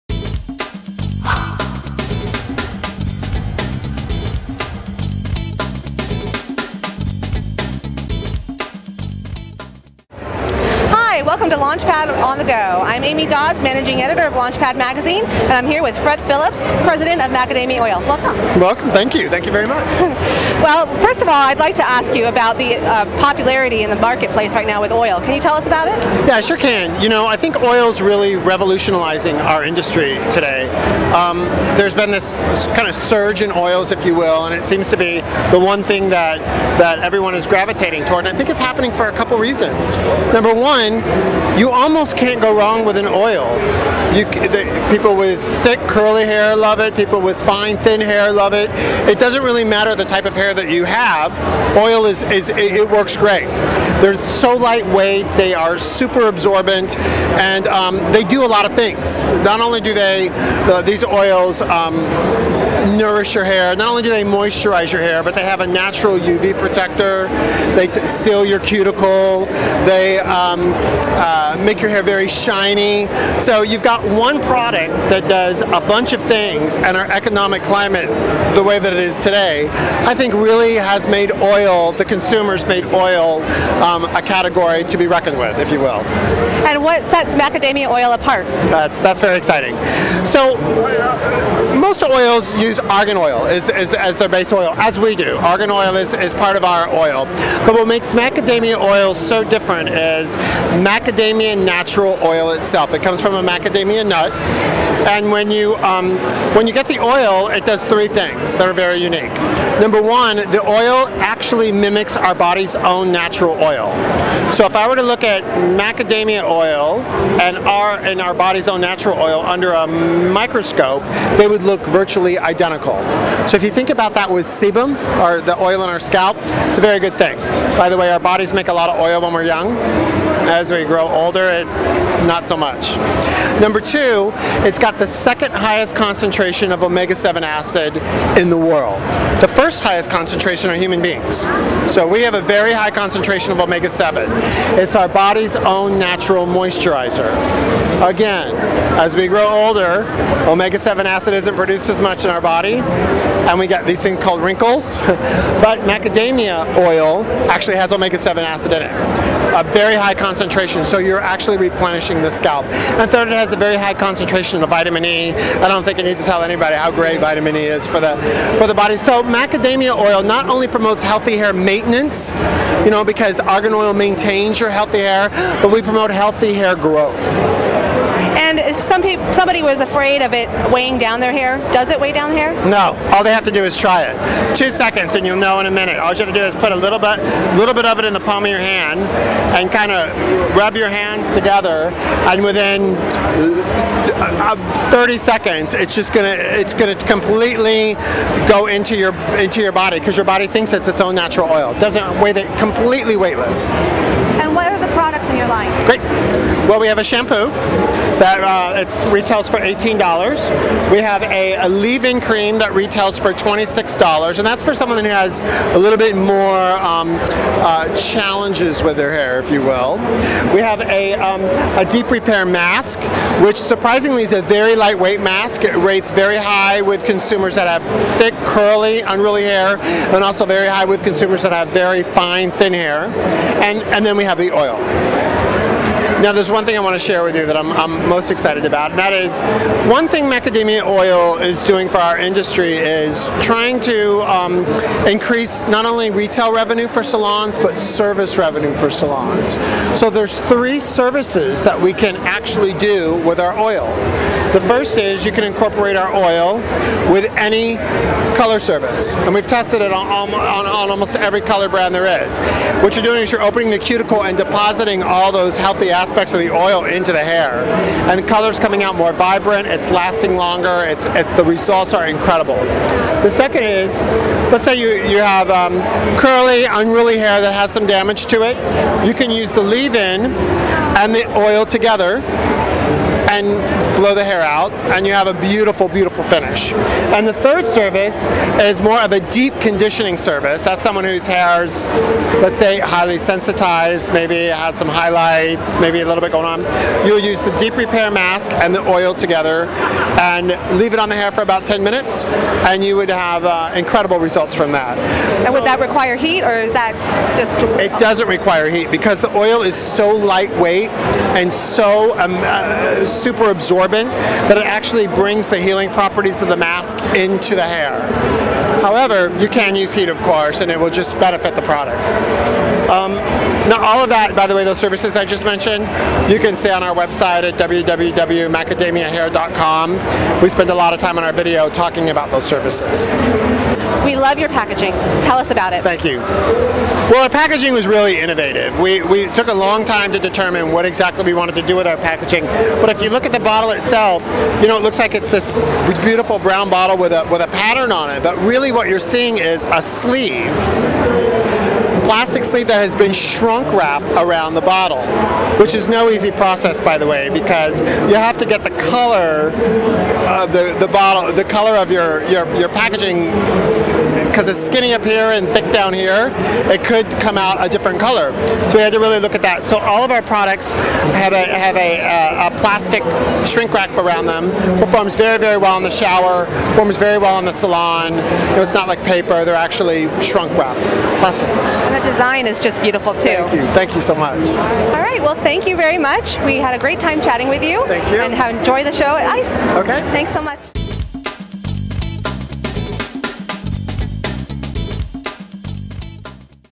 Launchpad on the Go: Macadamia Natural Oil
Fred Phillips of Macadamia Natural Oil chats about the current popularity of oils, what sets theirs apart, and their supercool packaging.